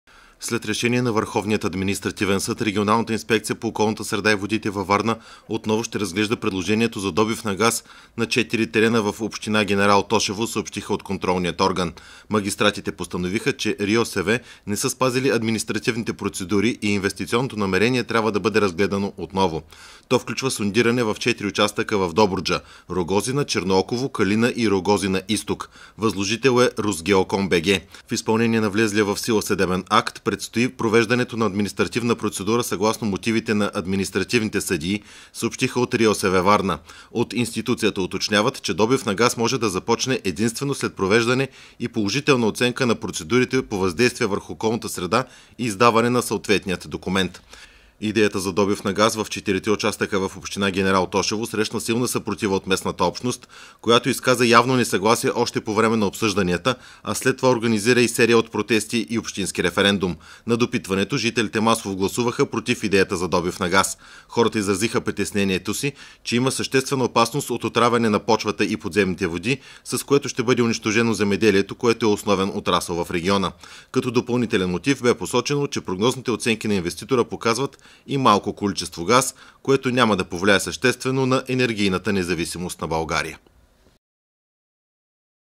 След решение на съда РИОСВ - Варна отново ще разглежда предложението за добив на газ в Добруджа
След решение на Върховния административен съд, Регионалната инспекция по околната среда и водите във Варна отново ще разглежда предложението на добив на газ на четири терена в Община Генерал Тошево, съобщиха от контролния орган. Магистратите постановиха, че РИОСВ не са спазили административните процедури и инвестиционното намерение трябва да бъде разгледано отново. То включва сондиране в четири участъка в Добруджа – Рогозина, Чернооково, Калина и Рогозина Изток. Възложител е „Русгеоком БГ“.
В изпълнение на влезлия в сила съдебен акт, предстои провеждане на административна процедура съгласно мотивите на административните съдии, съобщават от РИОСВ – Варна. От институцията уточняват, че добив на газ може да започне единствено след провеждане и положителна оценка на процедурите по въздействие върху околната среда и издаването на съответния документ.
Идеята за добив на газ в четирите участъка в Община Генерал Тошево срещна силна съпротива от местната общност, които изказаха явно несъгласие още по време на обсъжданията, а след това организираха серия от протести и общински референдум. На допитването жителите масово гласуваха против идеята за добив на газ. Хората изразиха притесненията си, че има съществена опасност от отравяне на почвата и подземните води, с което ще бъде унищожено земеделието, което е основен отрасъл в региона. Като допълнителен мотив бе посочено, че прогнозните оценки на инвеститора показват и малко количество газ, което няма да повлияе съществено за енергийна независимост на България.